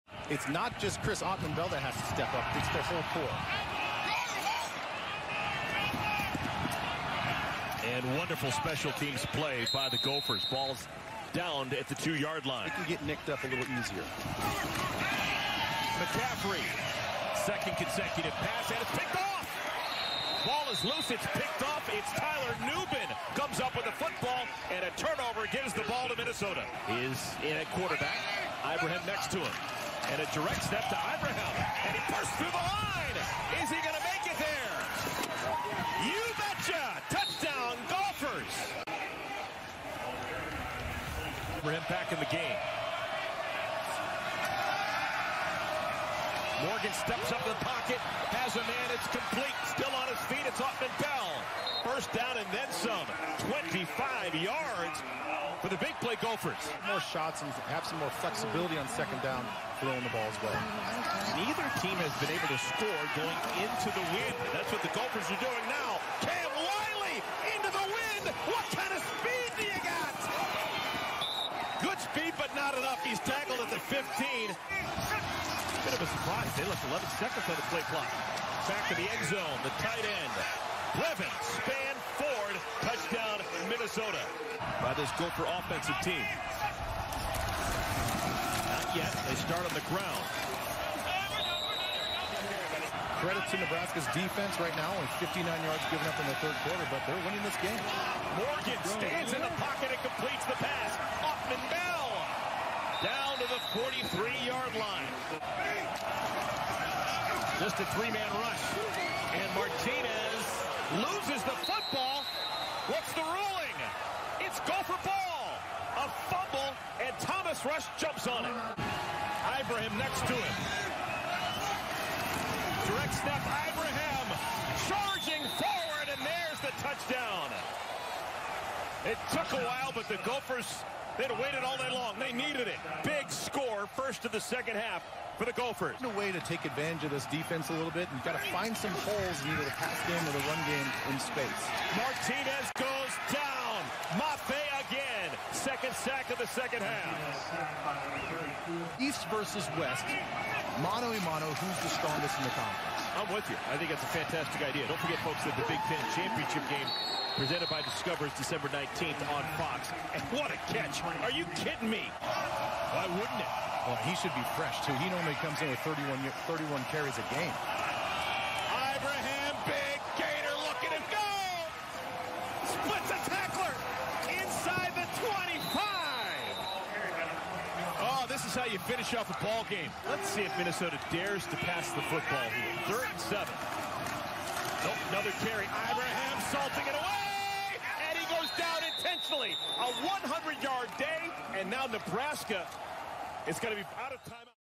Highlights: Gopher Football Defeats Nebraska, 24-17, in Lincoln
Mohamed Ibrahim ran for two touchdowns following Nebraska turnovers, and a Minnesota team missing nearly three dozen players to COVID-19 protocols or injuries left Memorial Stadium with a stunning 24-17 win on Saturday after being idle three weeks.